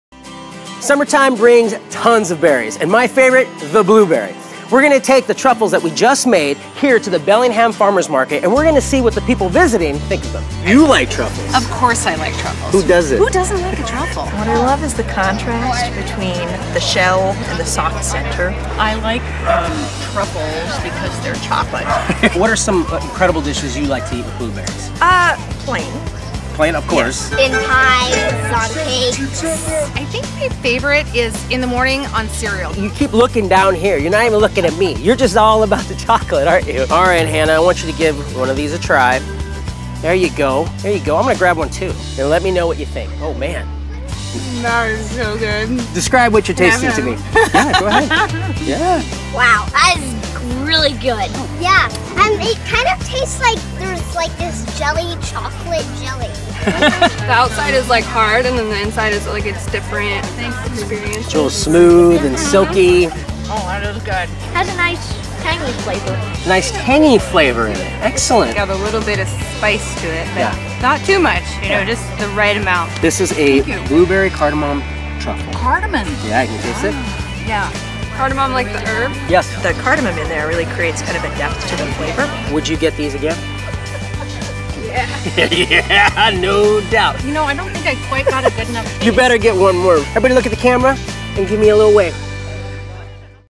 Truffle Sampling | Bellingham, WA | Washington Grown
Tomás takes to the market to see people's thoughts on the Blueberry Cardamon truffle.